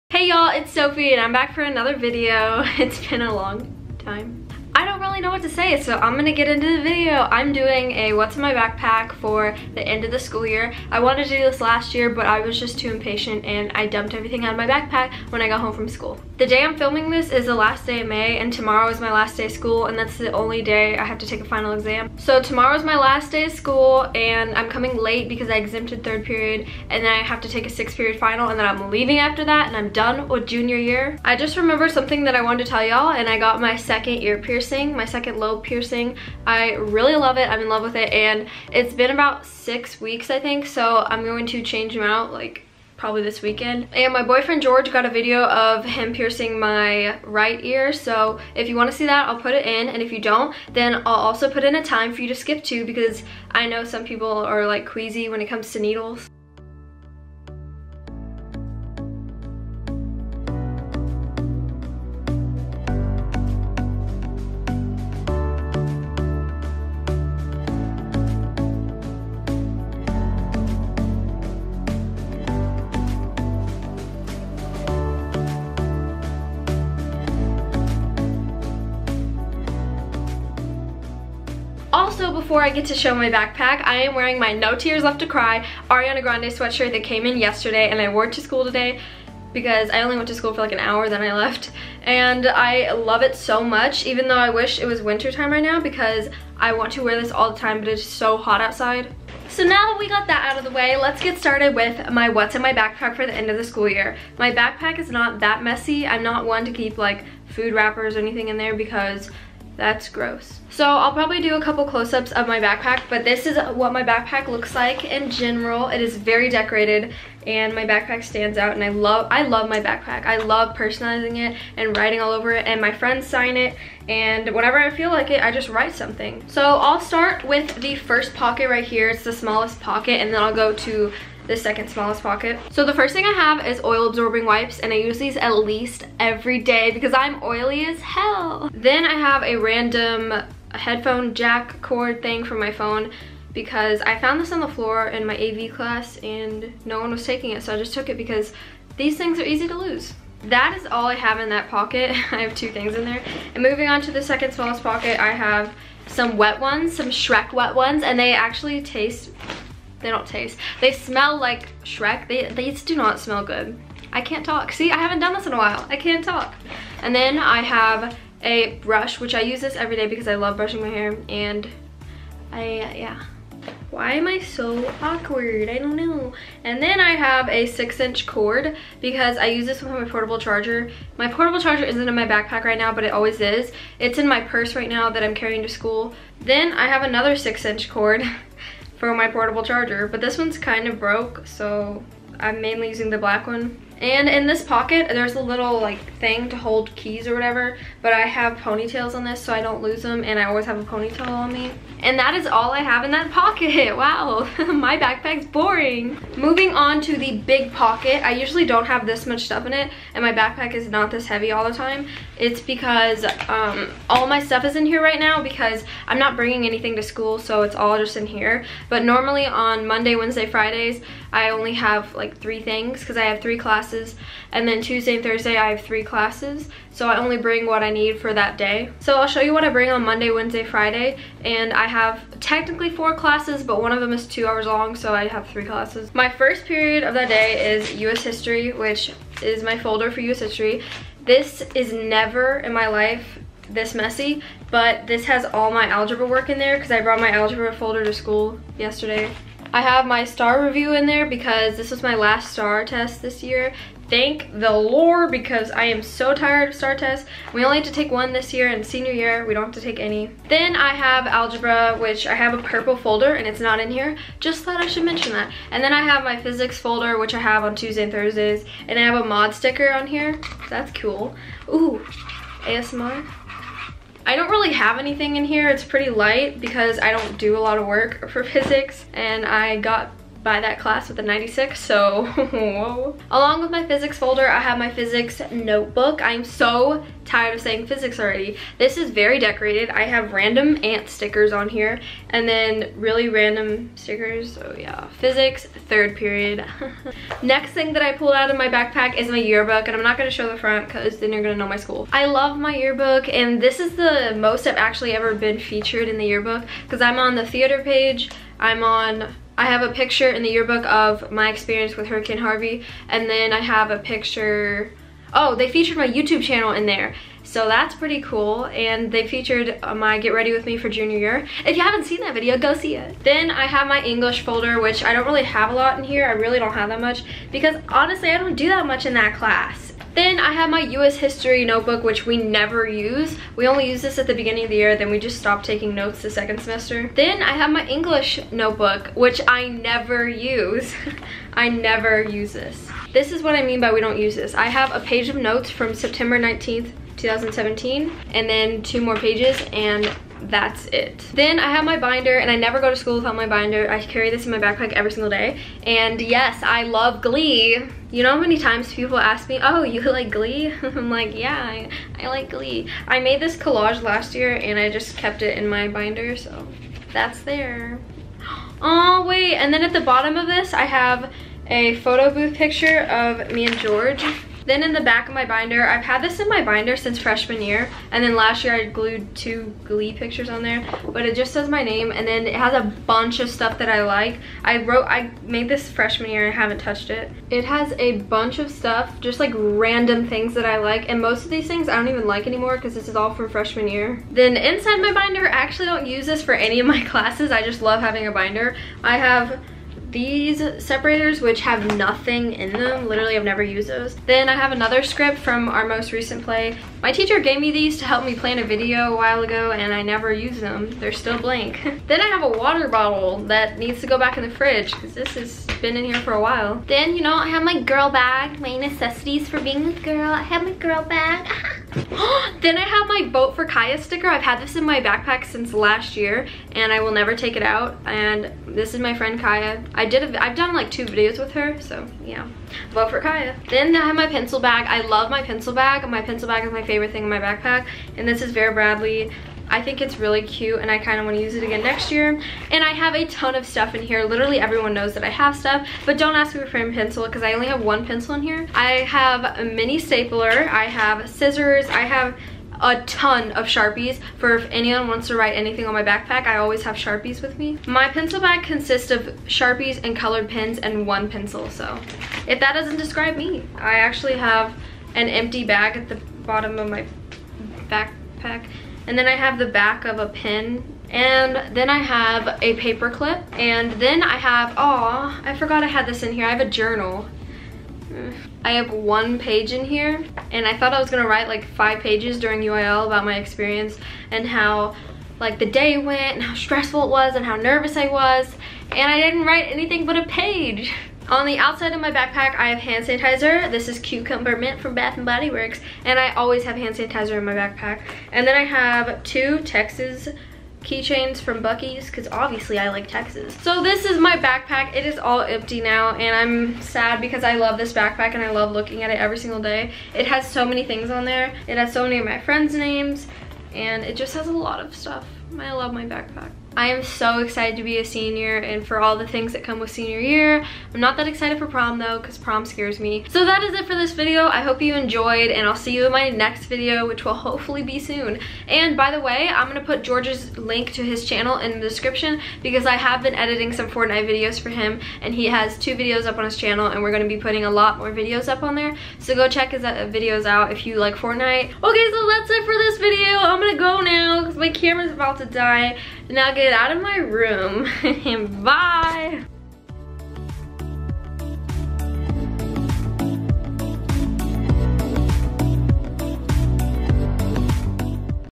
what's in my backpack? 2018 (end of the year)
hey y'all! it's been a whileee, but it is officially summer so that means more videos! here is my "what's in my backpack? end of the year", hope you enjoyed!